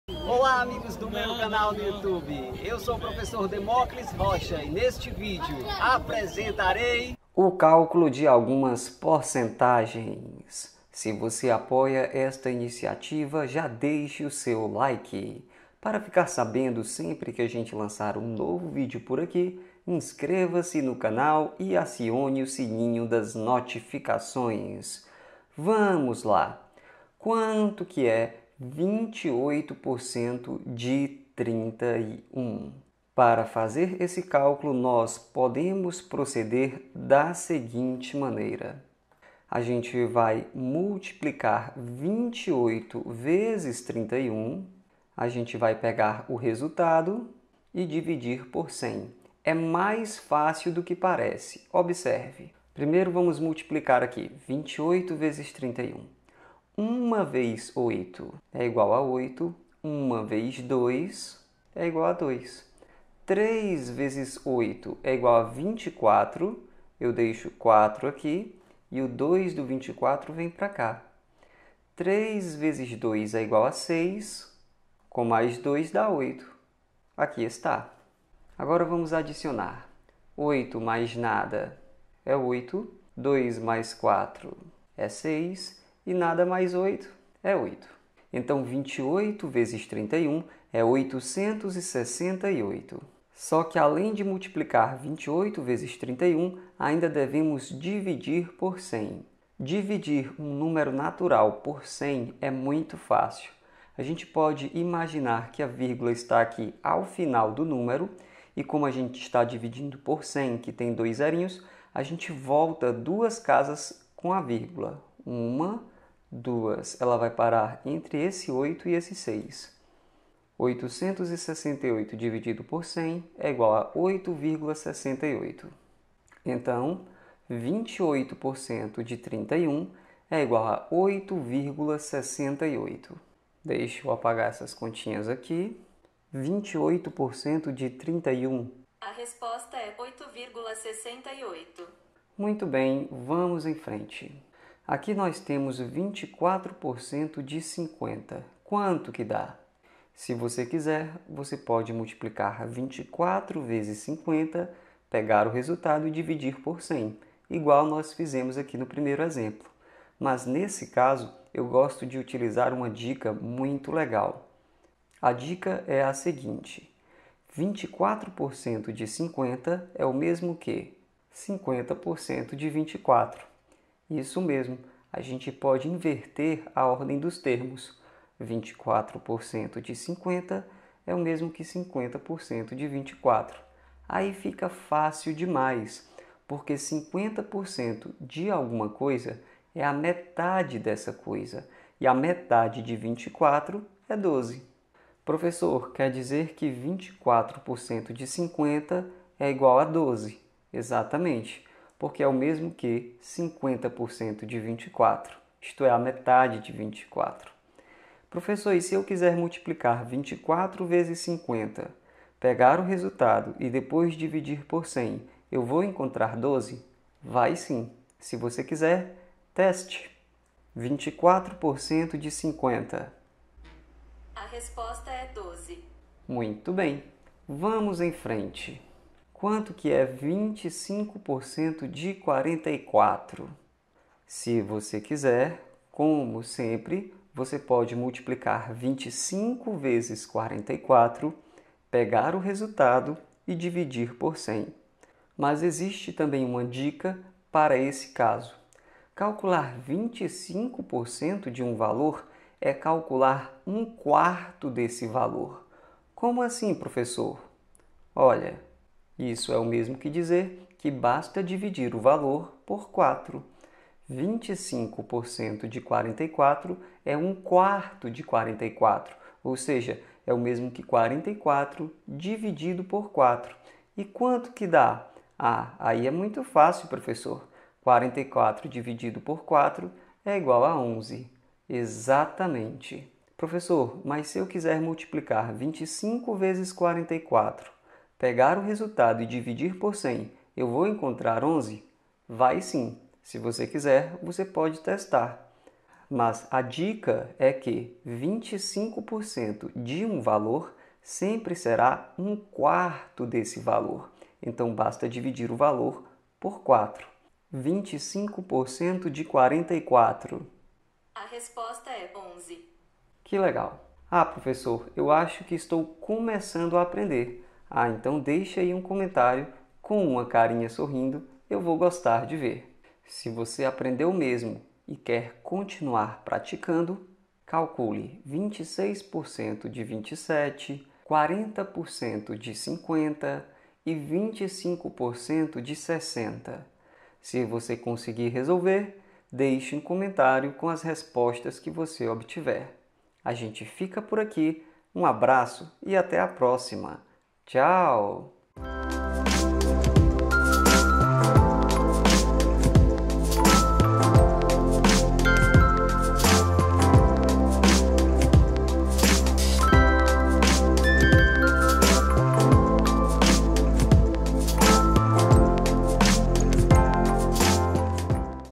PORCENTAGEM
o cálculo de algumas porcentagens.

Fortaleza - CE
60840974

Sony Vegas

Atube Catcher

MICROFONES

Lapela Sony: ECMCS3